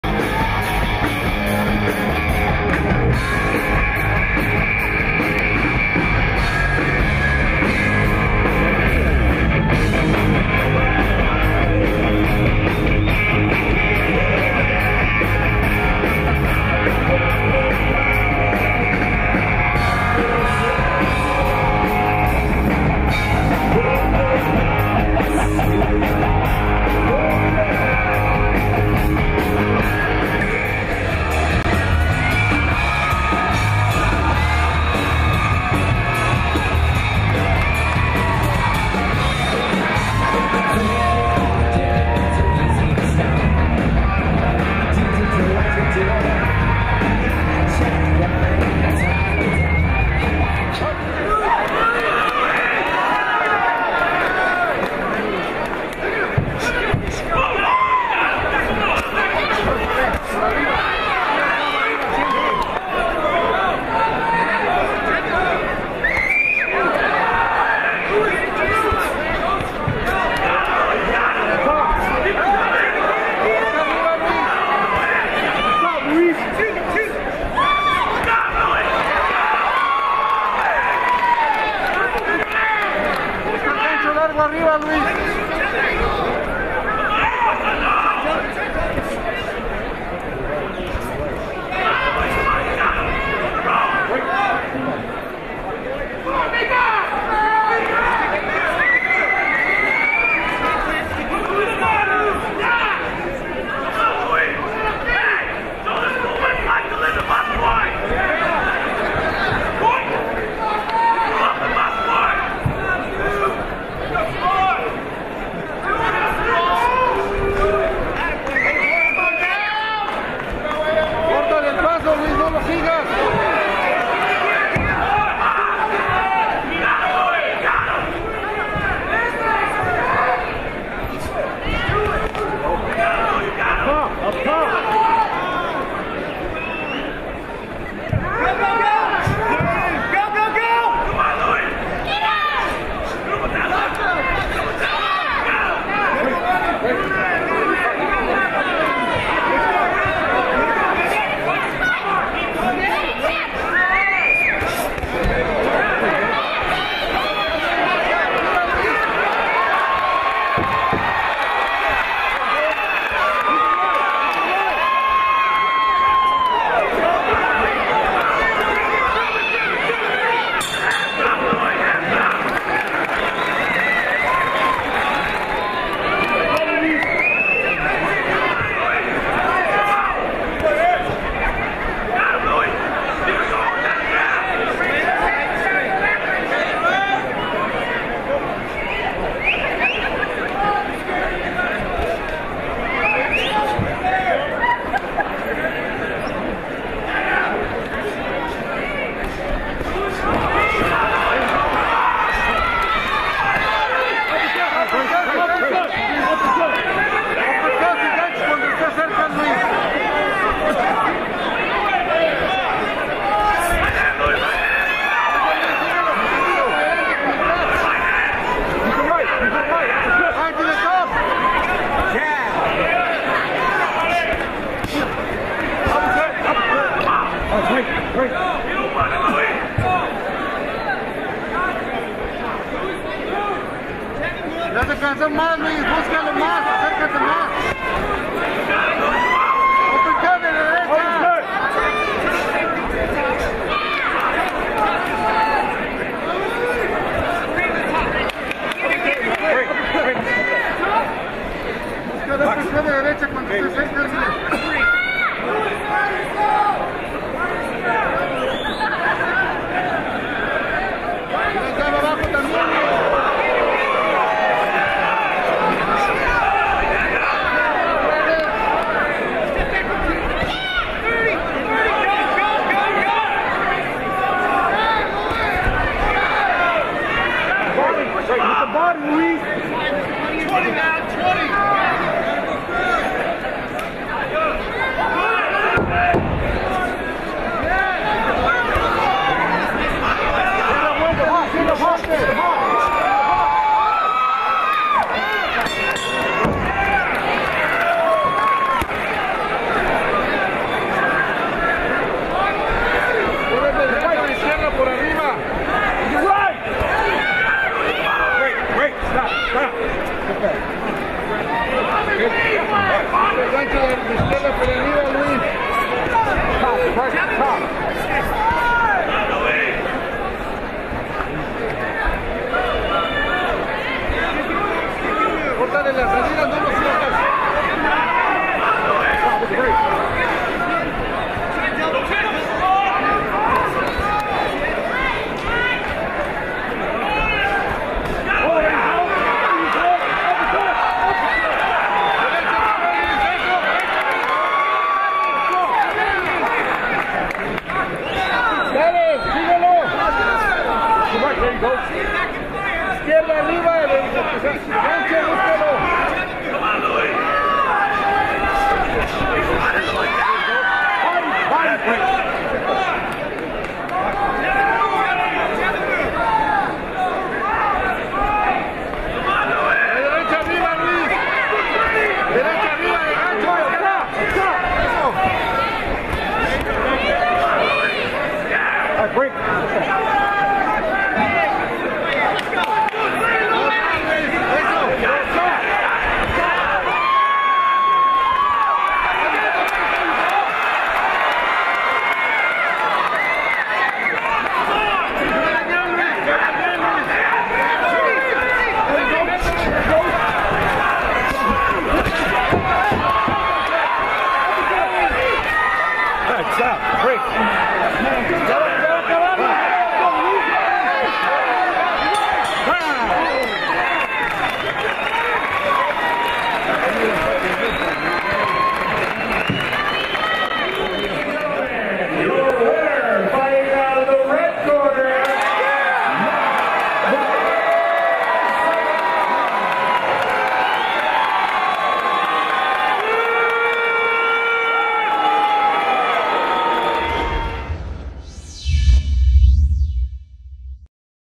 Matt Savant, Orange Co Fire Authority vs. Luis Garcia, Riverside PD, (175lbs)
Matt Savant, Orange Co Fire Authority vs. Luis Garcia, Riverside PD, (175lbs) SoCal Battle of the Badges, June 1, 2013, Agua Caliente Casino & Resort.